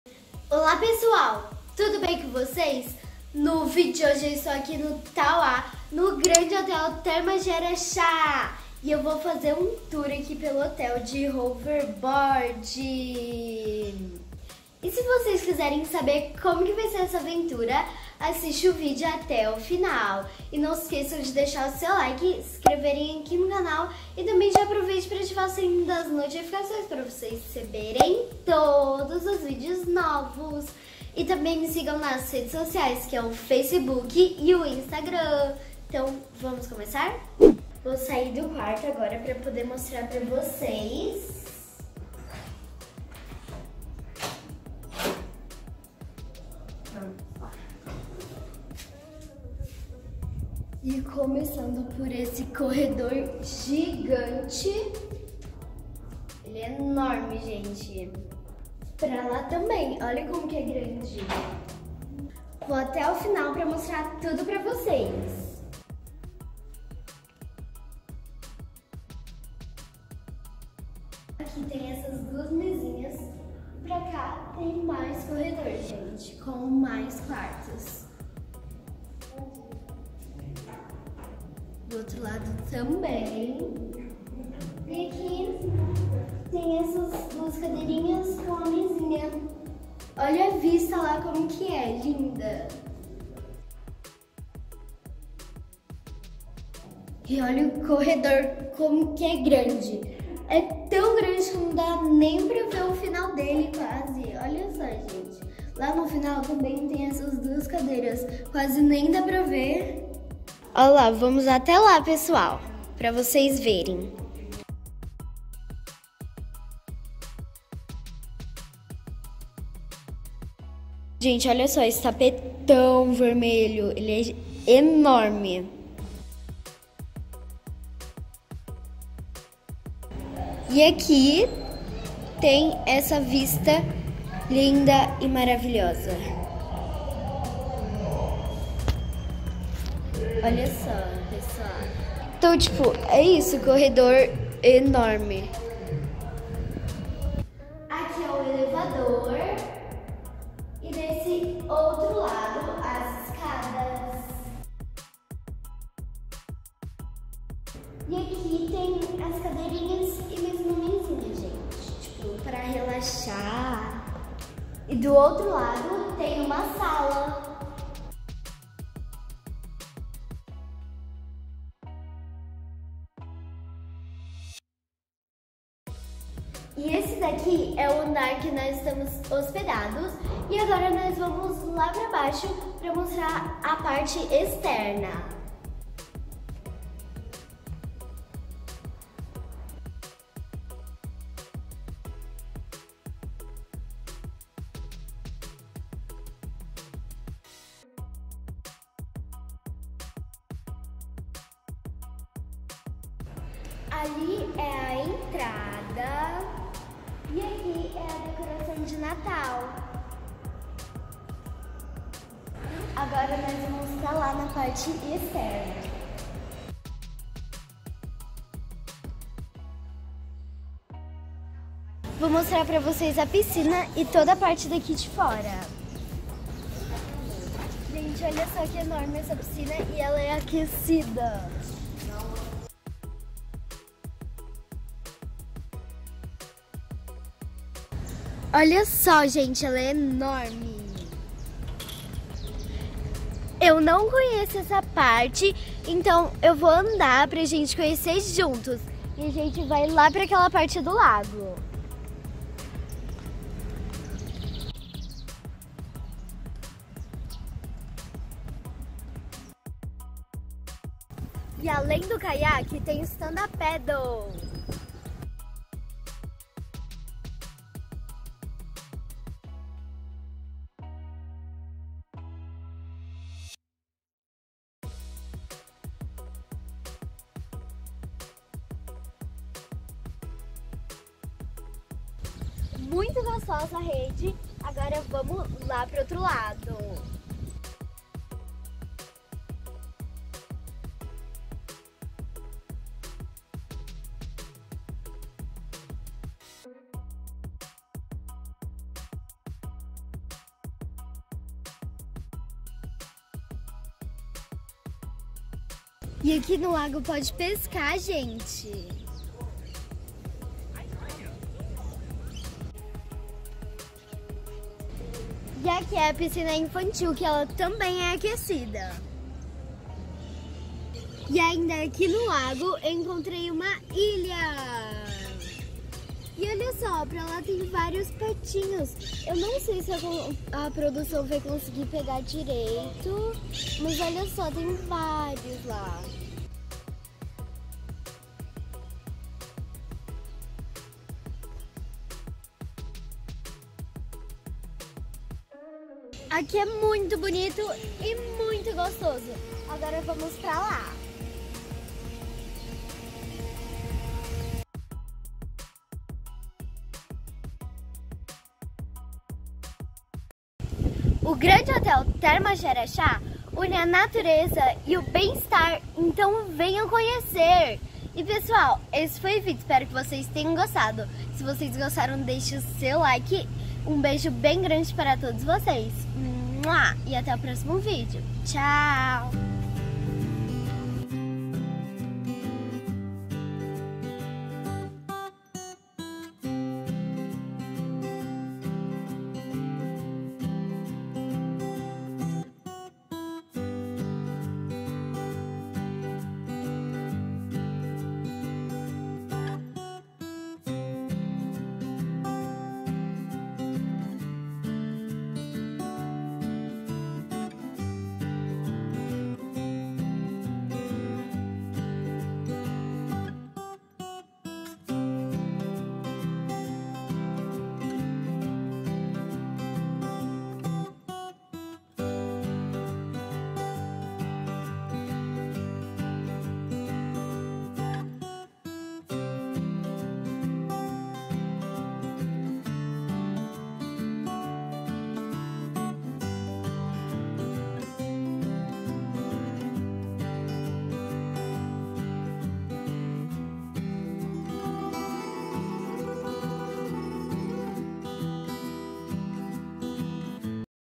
TOUR DE HOVERBOARD PELO MAIOR CASTELO DO BRASIL|Grande Hotel Termas de Araxá|Grazi Victoria Férias#2
Quem me acompanha no Instagram, sabe que eu fui convidada para fazer uma Oficina de Slime no Grande Hotel Termas de Araxá, que é o maior castelo do Brasil, e no vídeo de hoje eu vou fazer um tour pelo hotel de hoverboard!
Obs: Não visitamos a região das Termas e Restaurantes pq não é permitido andar de hoverboard nesses locais.